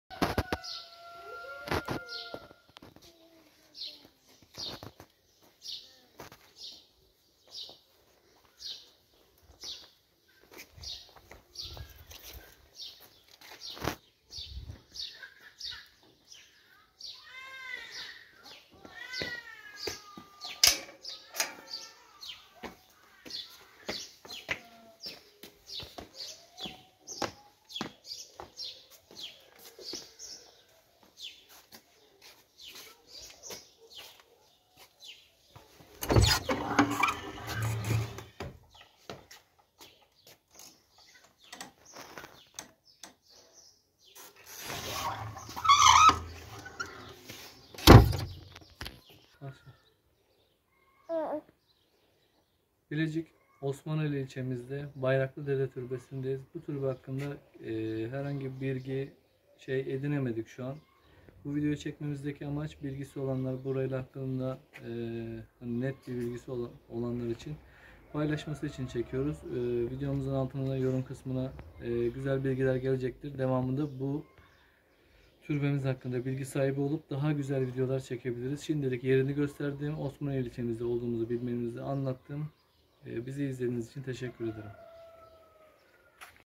Bayraklı Dede Türbesi - Bilecik / Osmaneli
Taşı Mermer, Toprağı Seramik, Yaprağı İpek, Şeyh Edebali Ocağı, Cihan İmparatorluğu'nun Doğduğu Ertuğrul Gazi Diyarı, Cumhuriyetin Temeli, Müdafaa-i Hukuk Cemiyetinin kurulduğu, Dirilişin Kuruluşun ve Kurtuluşun Şehri Memleketim Bilecik / Osmaneli ilçesi Bayraklı Dede Türbesi,

Konum;
Bayraklı Dede Türbesi